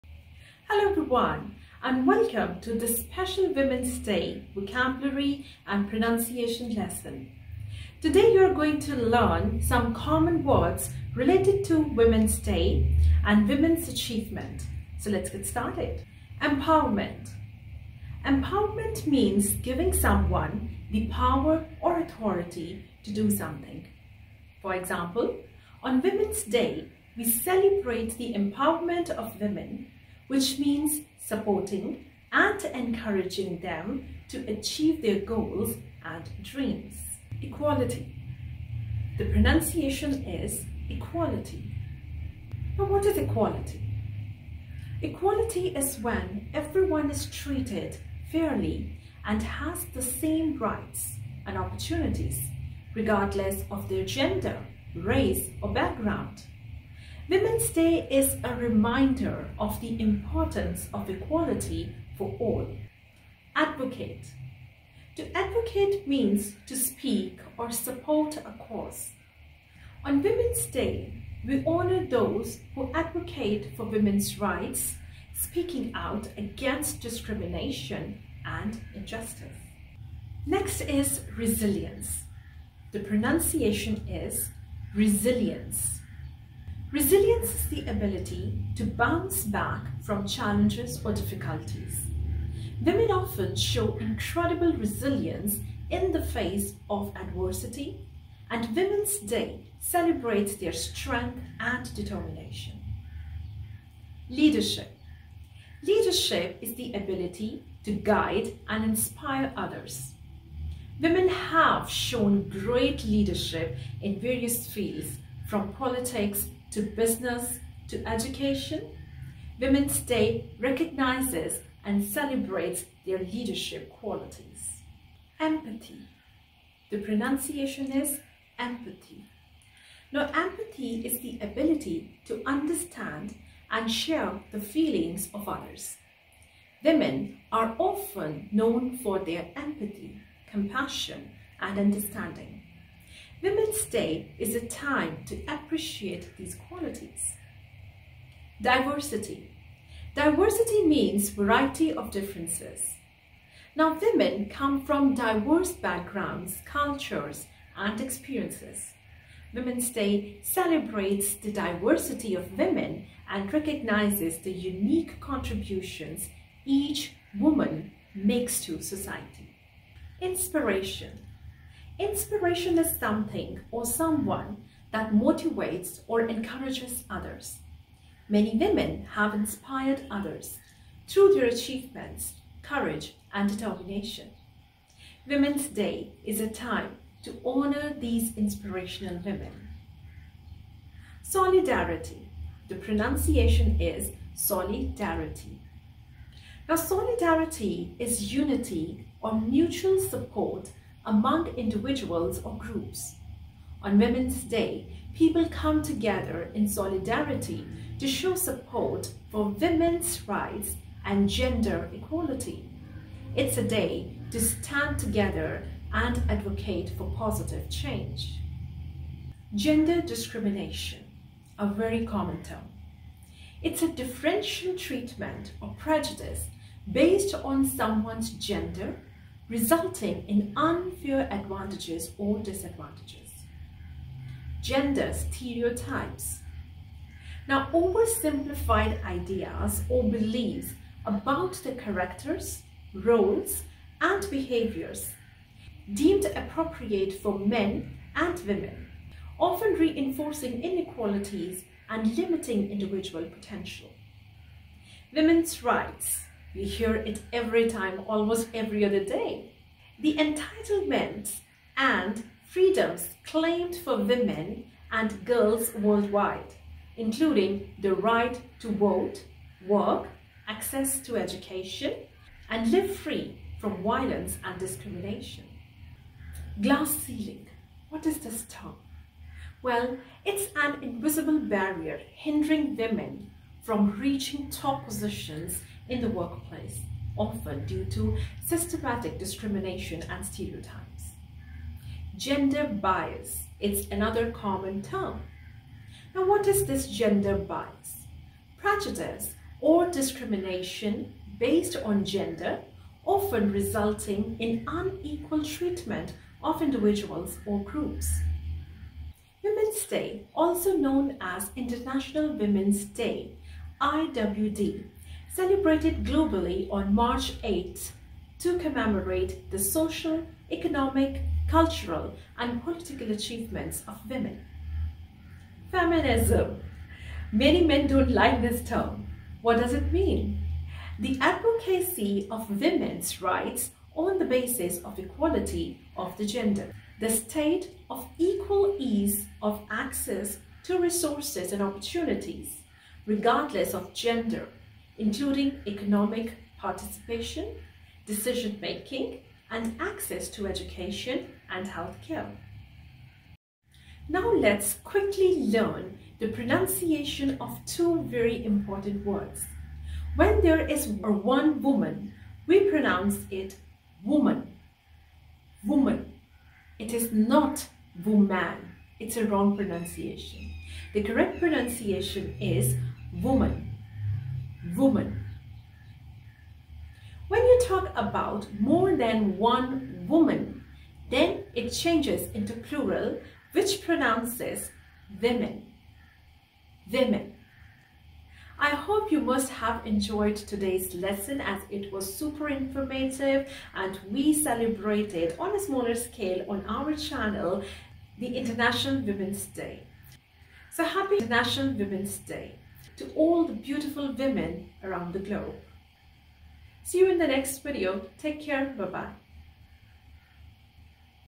International Women's Day 2024: Special Women's Day Vocabulary and Pronunciation Lesson
This video teaches you pecial Women's Vocabulary and Pronunciation Lesson.

So, watch the video till the end and improve your vocabulary and pronunciation.

Every relationship in this world is incomplete without a woman. She plays various roles like a loving mother, a caring sister, an understanding wife, and a lovable daughter. You embody all these qualities, and therefore, the world celebrates you. Happy Women's Day!

Let's celebrate Women's Day by promoting women's empowerment, breaking gender stereotypes, advocating for equality, fighting against gender bias, and promoting leadership, empathy, and diversity. Let's inspire and support one another to overcome gender discrimination and break the glass ceiling. We stand in solidarity with all the women fighting for their rights and power. Happy Women's Day to all the strong and amazing women out there!